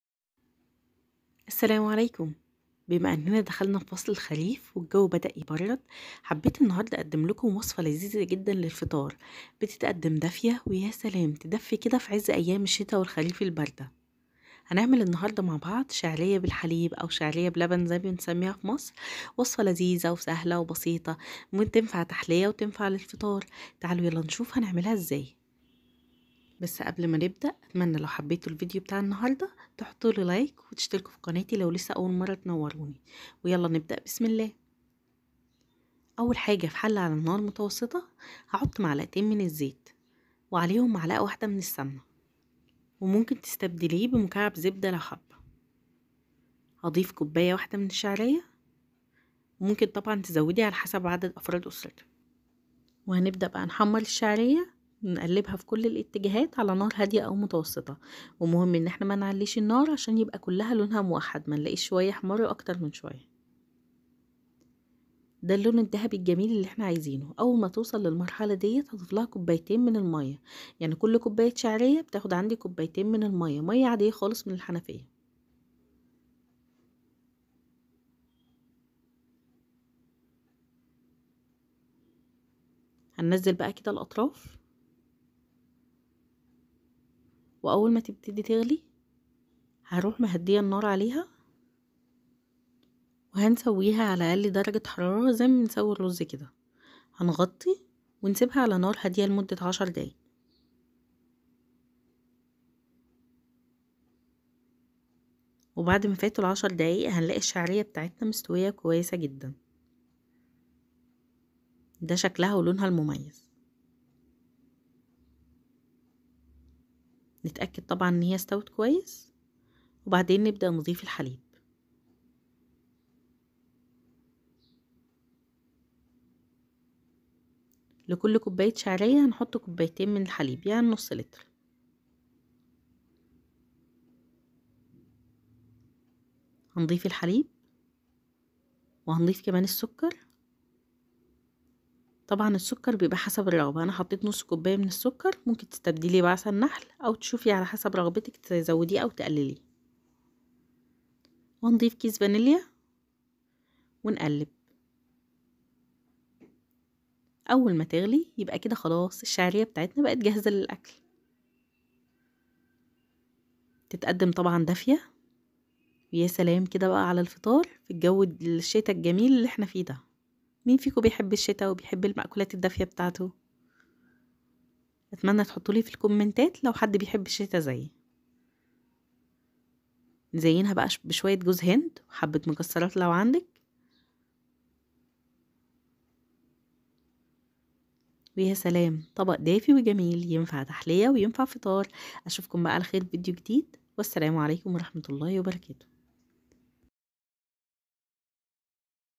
شعرية باللبن بأسهل وألذ طريقة / فطور شتوي دافئ وتحلية لذيذة milk vermicelli recipe
وصفة اليوم من ألذ وأشهى الوصفات الشتوية الدافئة
المكونات
كوب شعرية
٢ ملعقة زيت
١ ملعقة زبدة
٢ كوب ماء
٢ كوب حليب
فانيليا

مكسرات للتزيين

Today's recipe is one of the most delicious and warm winter recipes
Ingredients
1 cup vermicelli
 2 tablespoons of oil
 1 tablespoon butter
 2 cups of water
2 cups of milk
vanilla

nuts for garnish